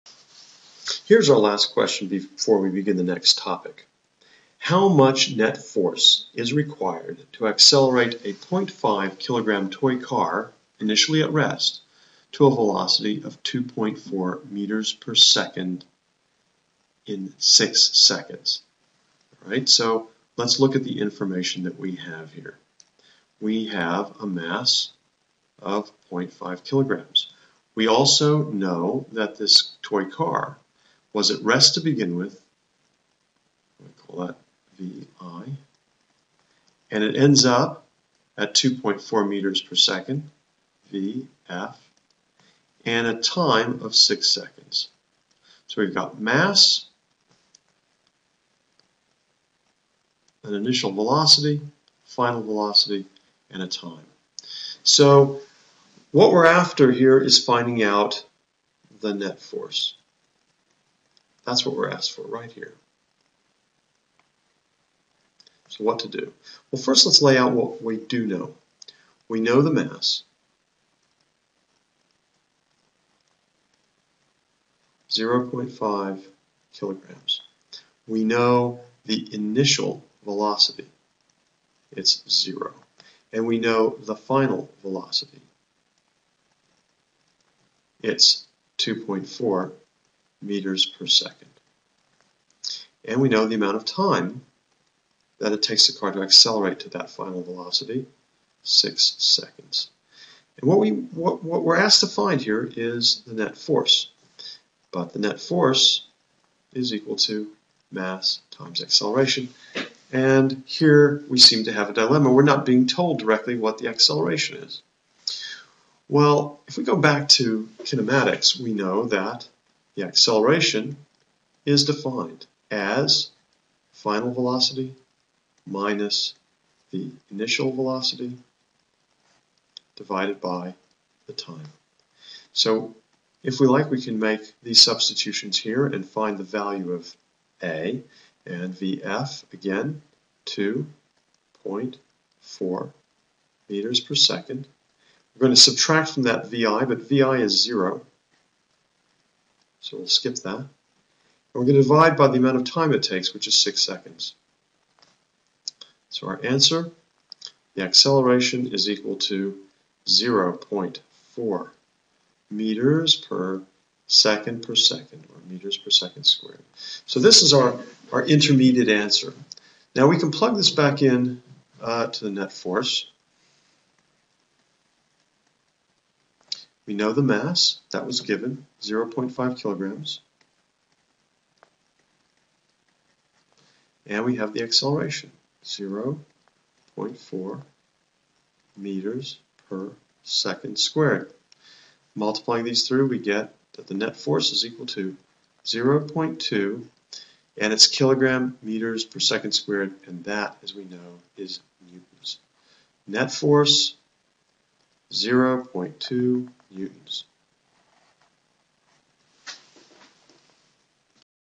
Dynamics - Newton's Second Law of Motion Q13
Slide text:  13 How much net force is required to accelerate a 0.5 kg toy car, initially at rest to a velocity of 2.4 m-s in 6 s? Students type their answers here [This object is a pull tab] Answer v = v 0 + at a = v-t = (2.4 m-s)-(6 s) = 0.4 m-s 2 Σ F = ma Σ F = (0.5 kg) (0.4 m-s 2 ) = 0.2 N given: m=0.5 kg v 0 =0 v=2.4 m-s t=6s Σ F=?